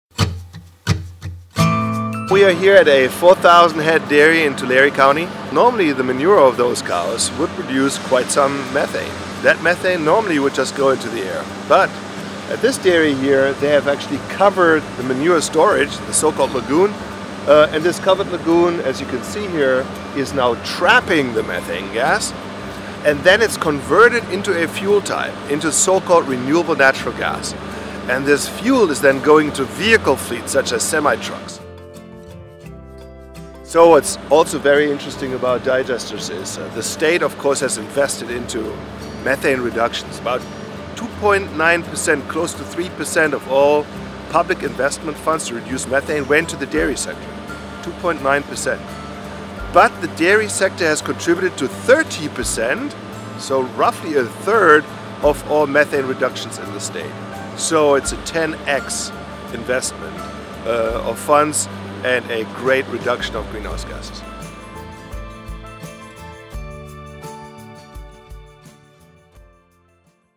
What are California Dairy Digesters?
Dr. Frank Mitloehner explains that dairy digesters reduce a lot of methane in California. They won't fit every dairy, but digesters are a worthwhile investment. Dairies have reduced 30% of the greenhouse gasses mitigated in the California Climate Investment Initiative with less than 2% of state funding.